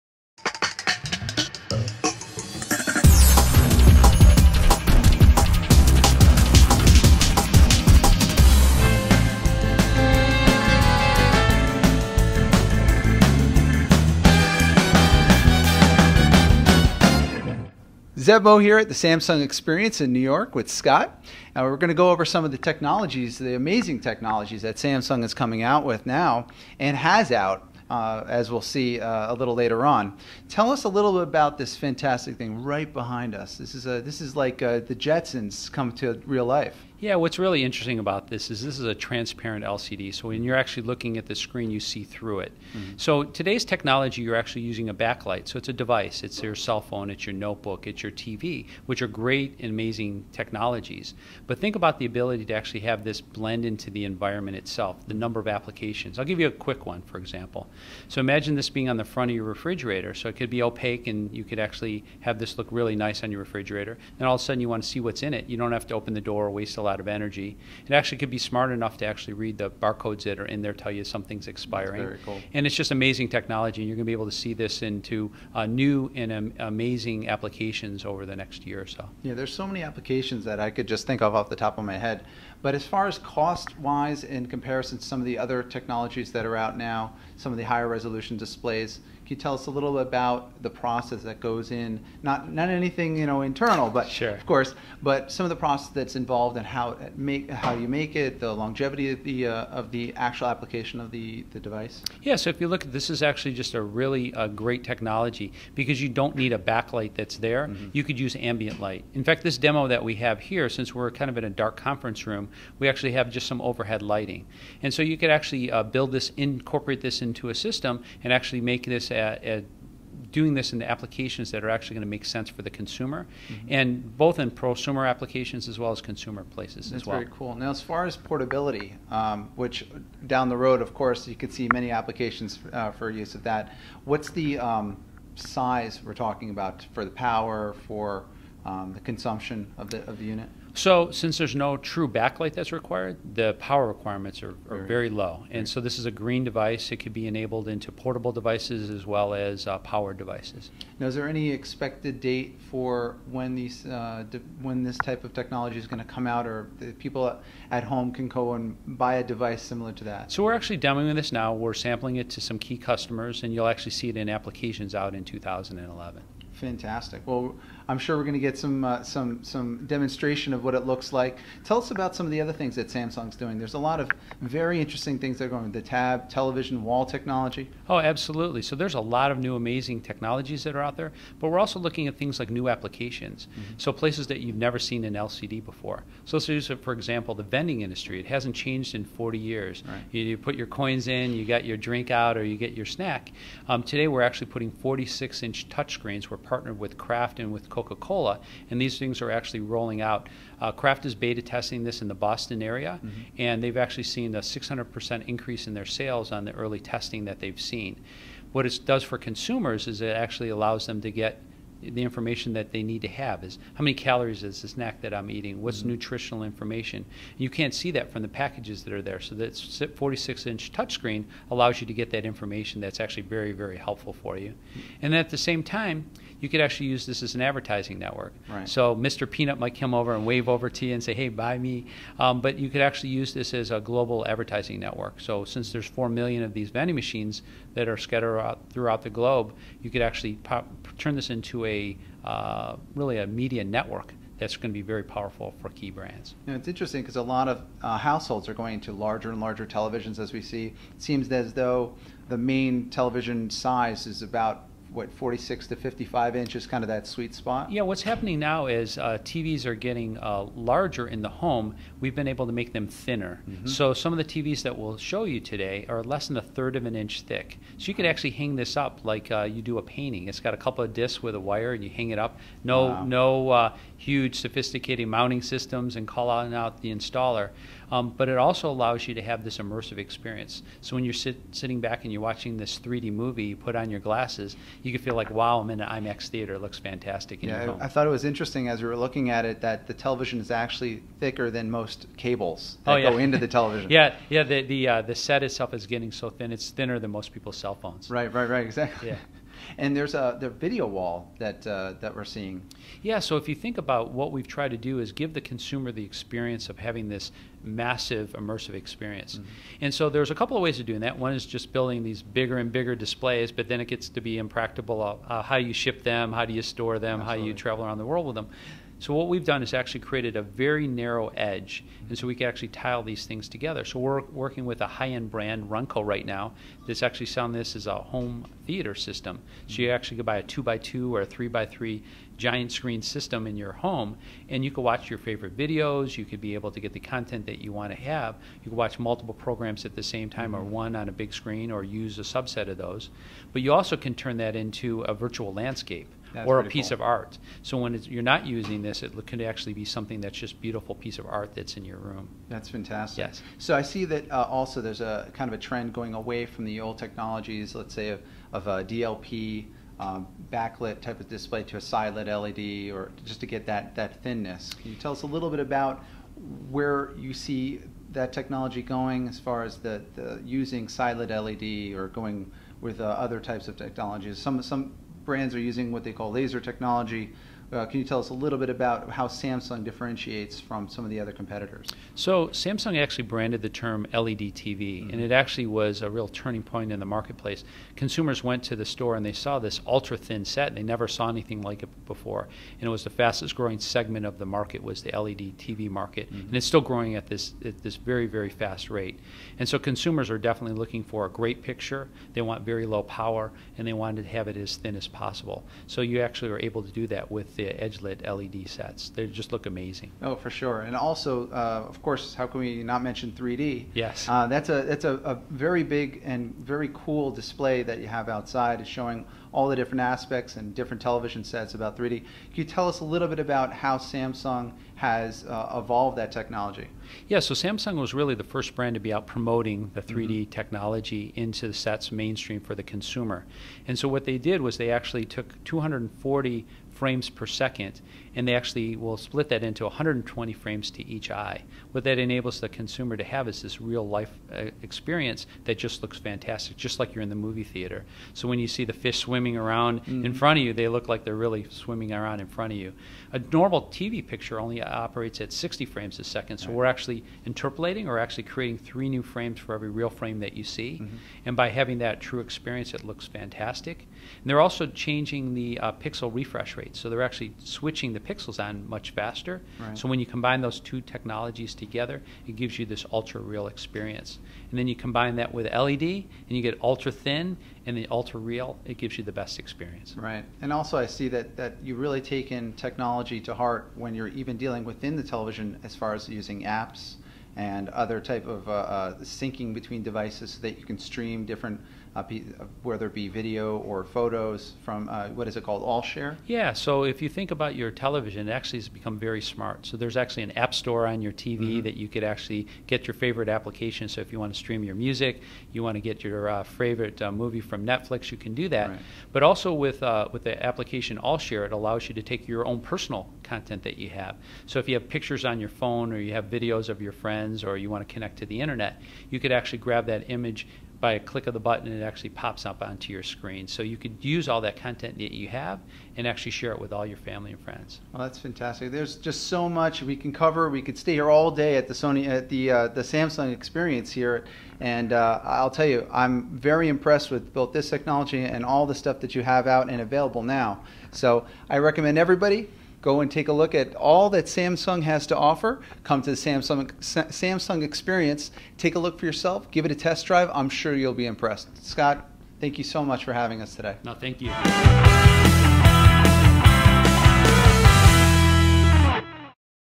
Samsung transparent LCD display window technology
Zev Mo (@ZevMo) and Ramon B. Nuez Jr. (@RamonBNuezJr) headed down to the Samsung Experience at Time Warner Center, in New York, to check out some of the phenomenal display technologies that are currently available, as well as some future products not yet on the market.

We had the wonderful opportunity to speak with Scott Birnbaum, Vice President of LCD Business, Samsung Semiconductors, about their newest transparent display, thin LED HDTV's, AllShare, video walls, touchscreen vending machines, and 3D technology.